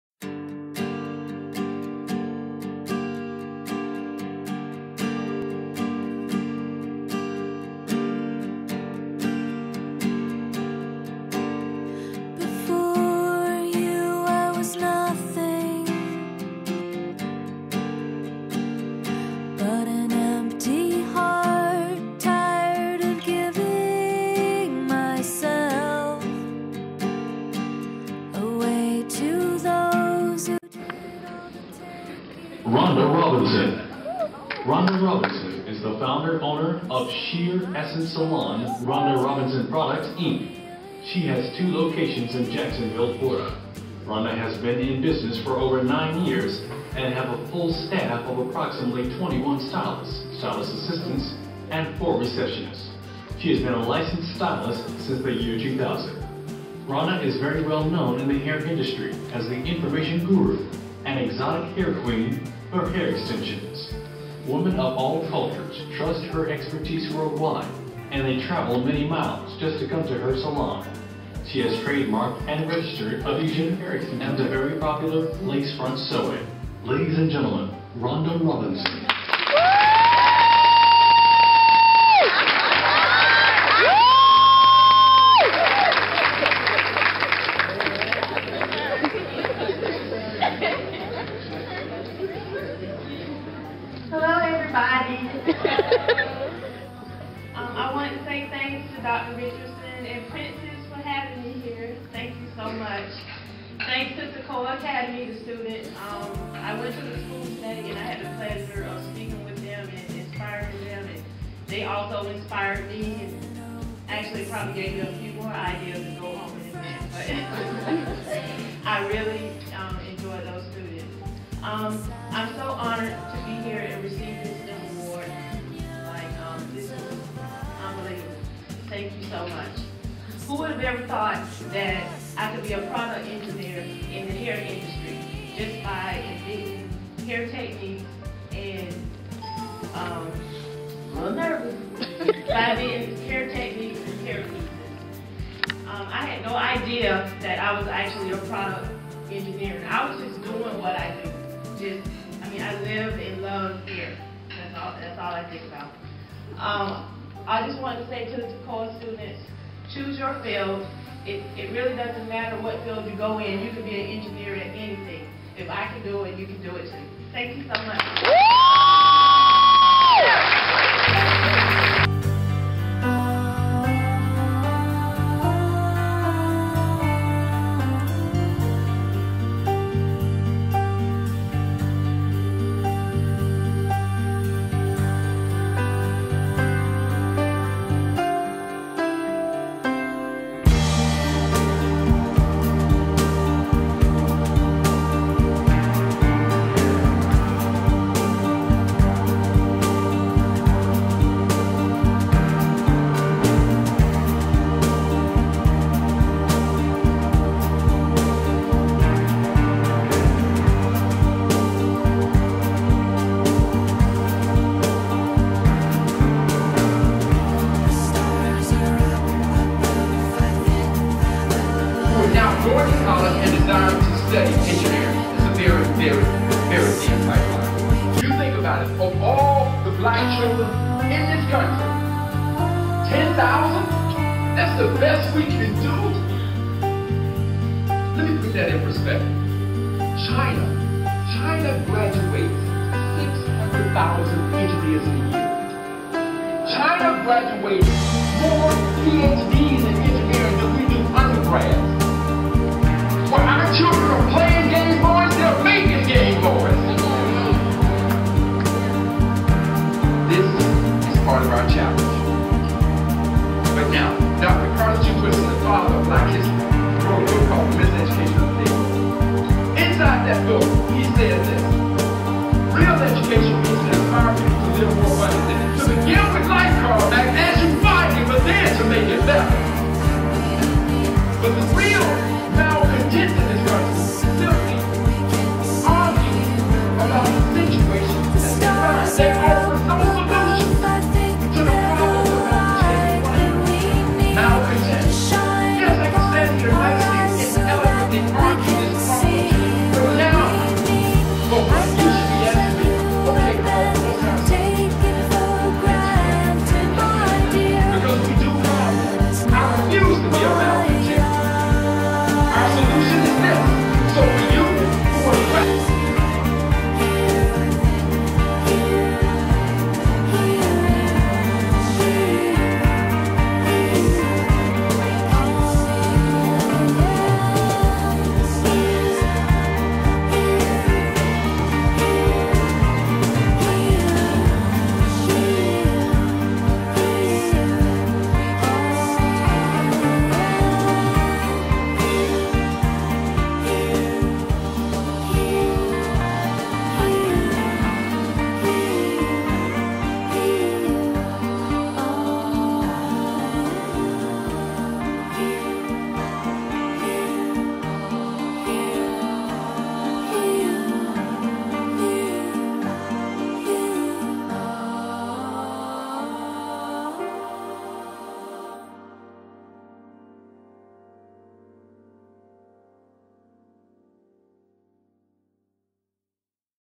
Rhonda Robinson “Weave Genius”Receives STEM Award Texas Lace Frontal Sewin™️ & Braidless Sewin™️
~ Video by Rhonda Robinson, Renowned Hair Stylist Rhonda Robinson receives Inspire STEM (Science Technology Engineering & Mathematics) Award by Tekoa Academy of Accelerated Studies on December 6th, 2012 for Product Engneering. This was an event I will always remember. I cherished each and every moment. Tekoa's 2nd Bi-Annual Scholarship & InSpire Awards Gala. A Red Carpet Event. Event was held @ The Holidome -Beaumont, TX. Director: Dr. Paula Richardson. Keynote Speaker: Dr. Carl B. Mack.